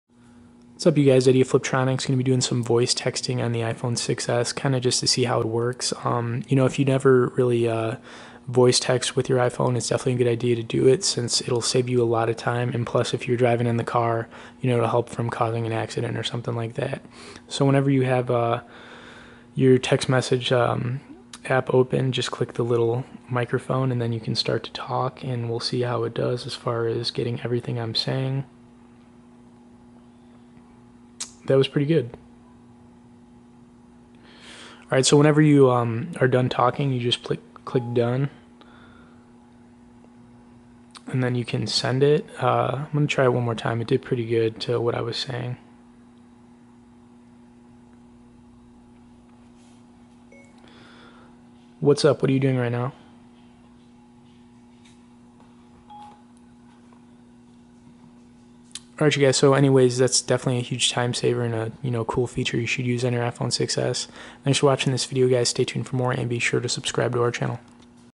Iphone 6s: Text Messaging Using Your Voice - Fliptroniks.com
Best Iphone 6s / 6 Accessories
Best Iphone 6s Plus Accessories

Iphone 7
Iphone 7 Plus

Text messaging using your voice on your iphone 6s is nothing new, but its kind of cool to see how it works if your not using it much. Simply open the text message app and click the microphone button and start speaking. After doing a couple of tests it pretty much got everything I was saying pretty well, and this is an absolutely great tool for when your driving or don't have time to type.